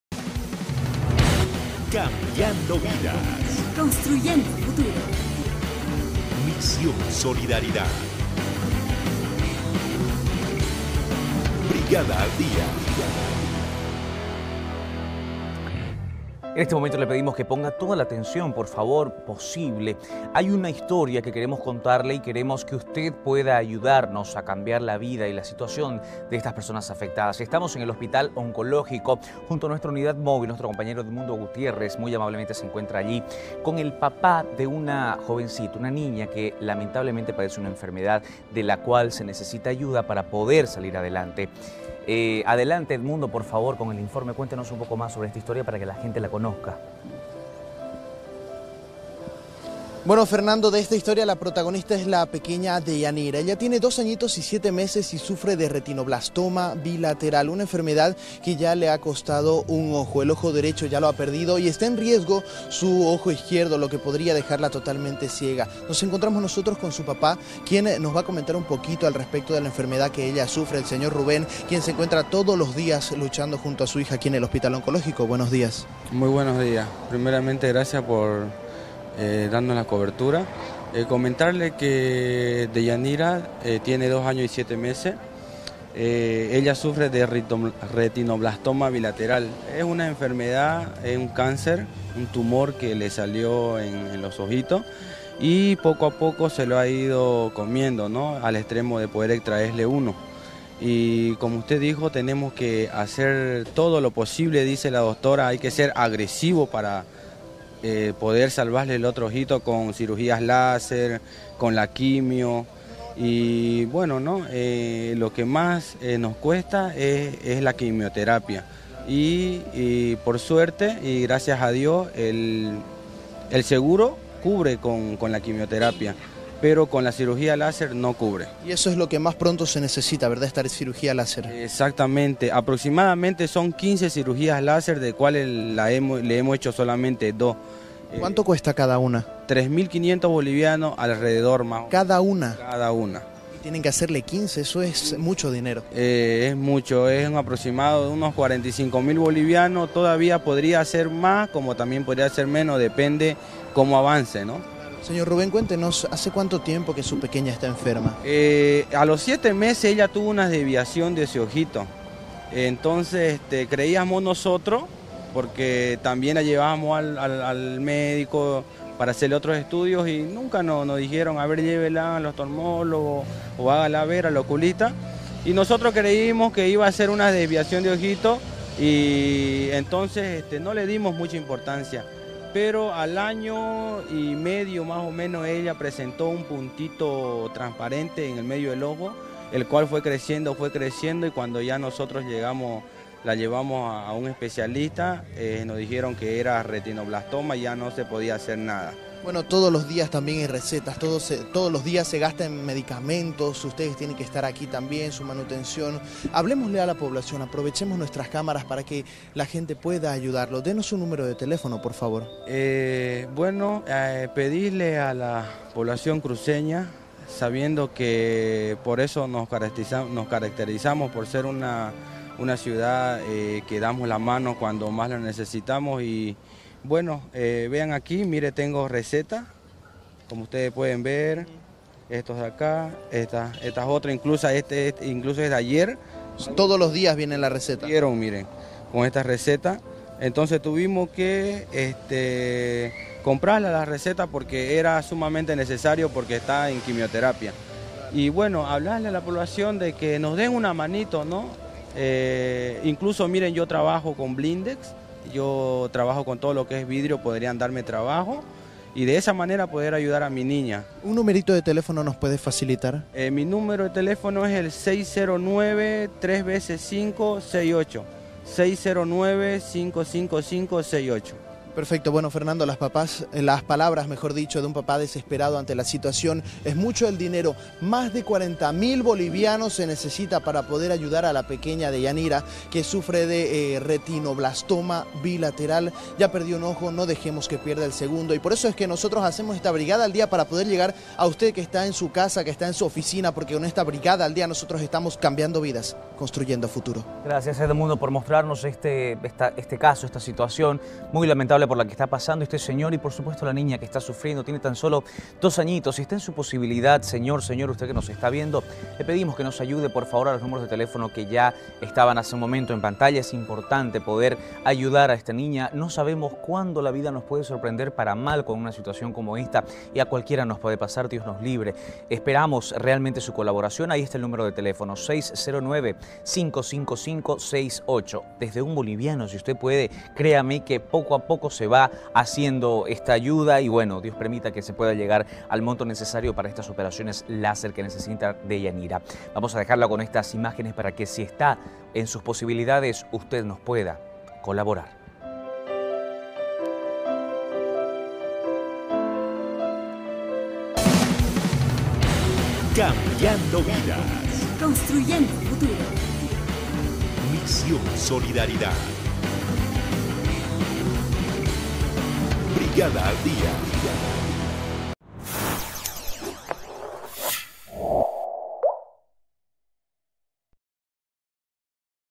Niña de dos años que padece de retinoblastoma bilateral necesita ayuda
En el espacio de ''Brigada al día'' le mostramos en detalle el caso de la niña de dos años, Deianira quien padece de retinoblastoma bilateral, por lo que ha perdido un ojo y tiene en riesgo el otro. La menor necesita la ayuda de la población.